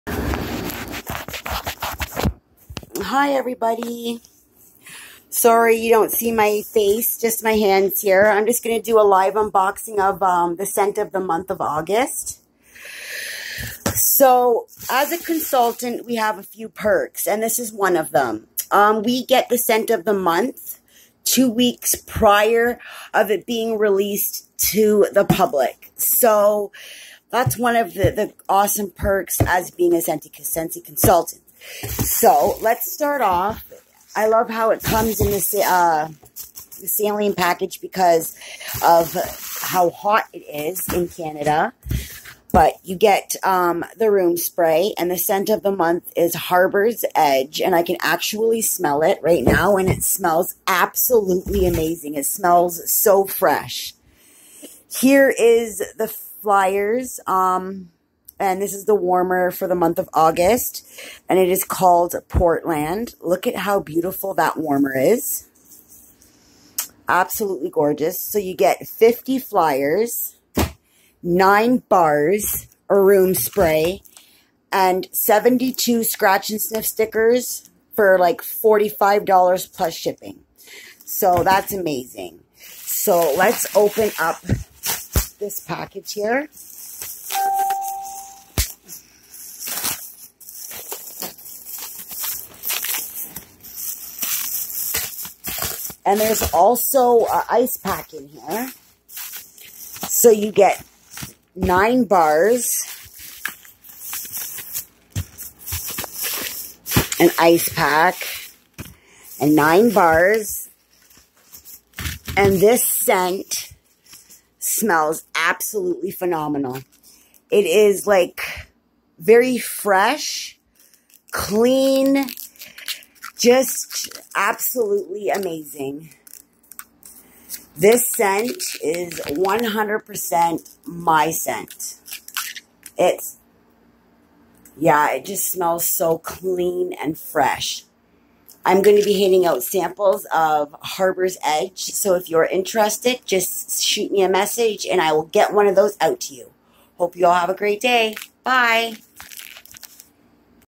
Scent of the month for August
This scent you guys, is absolutely amazing.  So clean and fresh smelling.